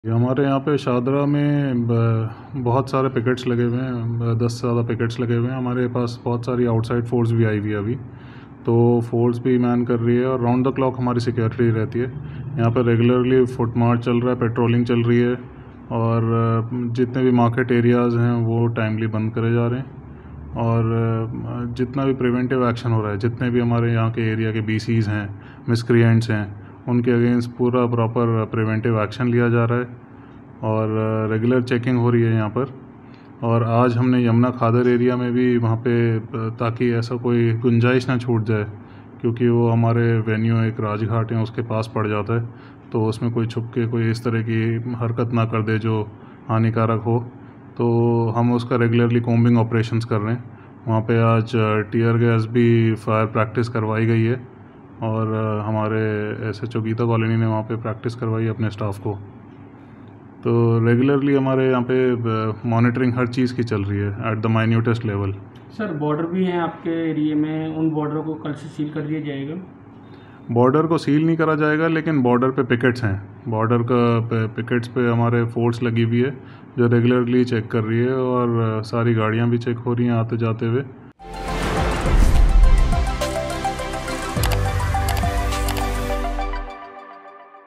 G20 Meeting Delhi से पहले Delhi Police की क्या तैयारियां हैं, यहां जानिए । Bharat Mandapam
Days ahead of the G20 Leaders’ summit in Delhi, Delhi police and security forces are on their toes. Delhi Police conducted a tear gas exercise on September 06. The G20 Leaders’ Summit will be held in Bharat Mandapam, New Delhi on September 9-10. DCP Incharge, Shahdra, Harsh Indora informed that the security forces are working round the clock and that foot march and patrolling is being conducted regularly.